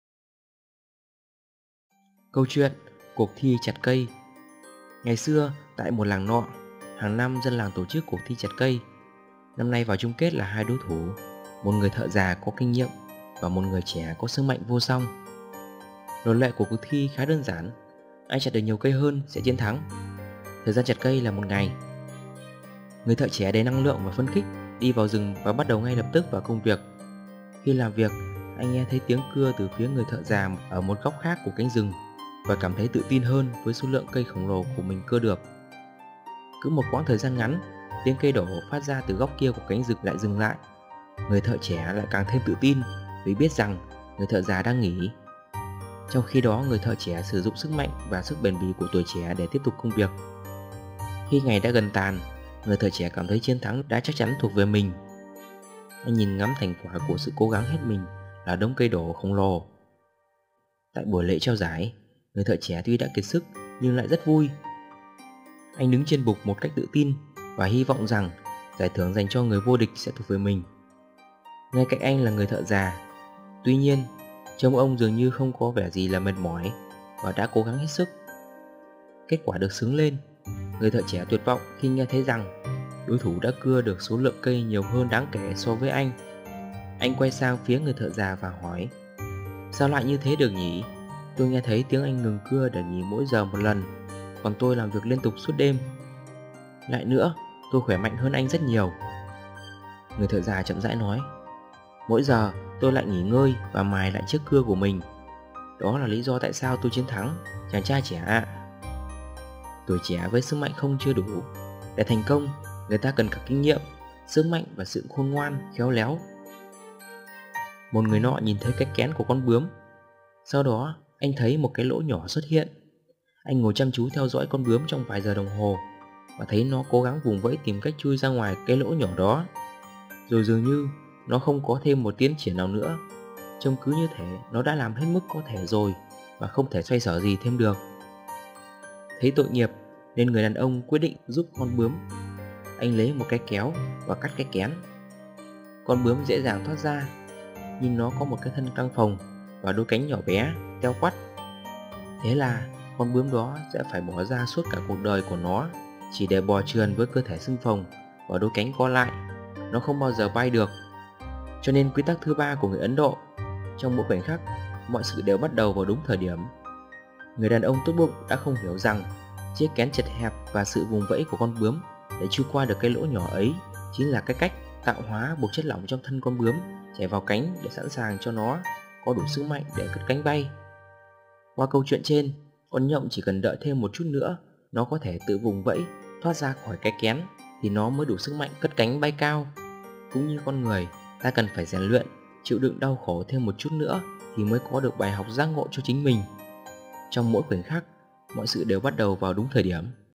Câu chuyện Cuộc thi chặt cây và Kén bướm
Ý nghĩa :
Tuổi trẻ với sức mạnh không chưa đủ. Để thành công, người ta cần cả kinh nghiệm, sức mạnh và sự khôn ngoan, khéo léo.
Trong mỗi khoảnh khắc mọi sự đều bắt đầu vào đúng thời điểm